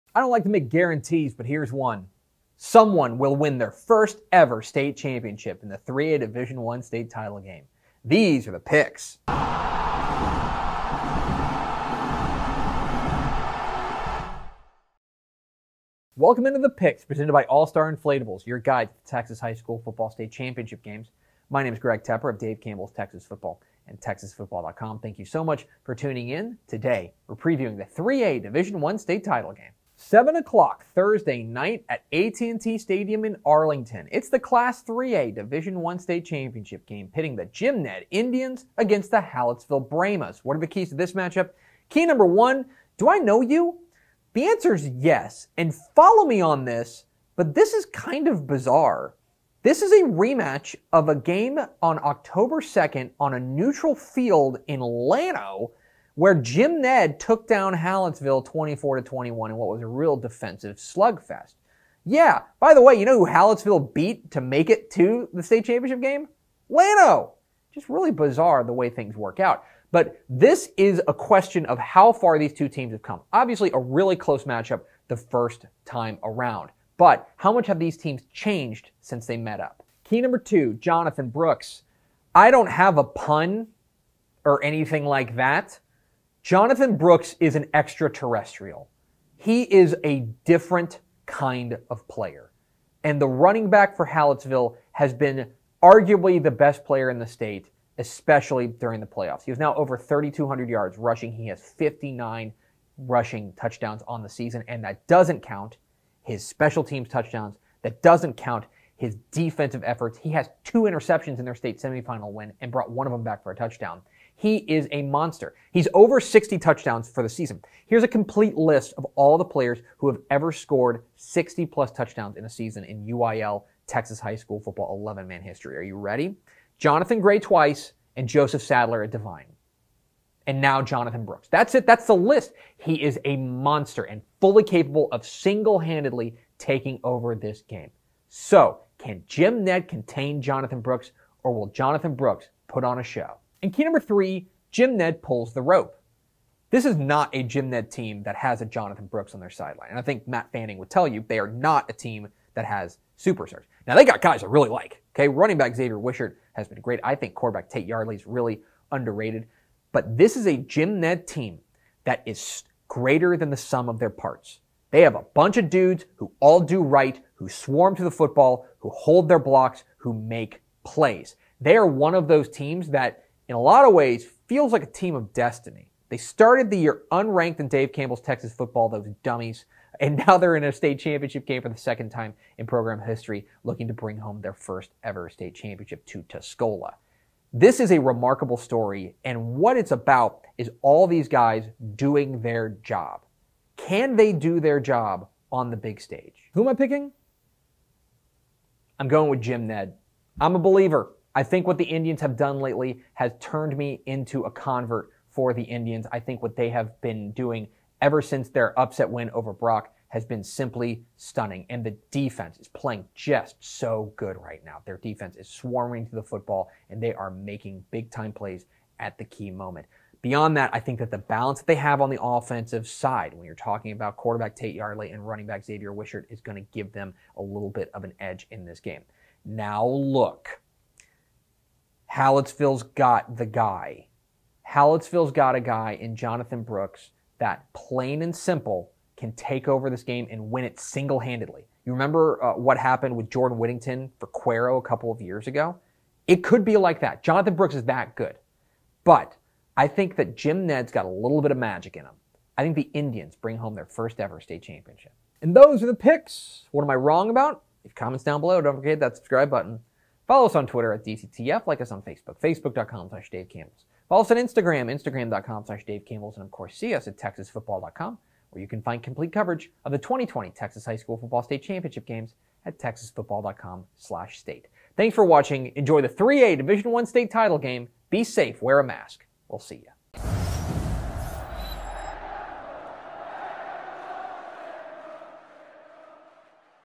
Jim Ned vs Hallettsville: 2020 UIL 3A DI Texas High School Football Championship Preview
Tepper makes his predictions for the 2020 3A DI State Championship game between Jim Ned and Hallettsville.